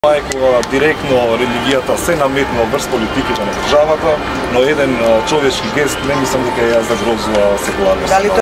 Statement by Fari Hiseni on Silence with Prayer in Assembly for Srebrenica Genocide Victims
Fari Hiseni from coalition "Vlen" responds to reporters questions to comment whether the one minute silence in Assembly during event for victims of Srebrenica disturbs the secularity of the state. With the Constitution religion and state affairs are separate.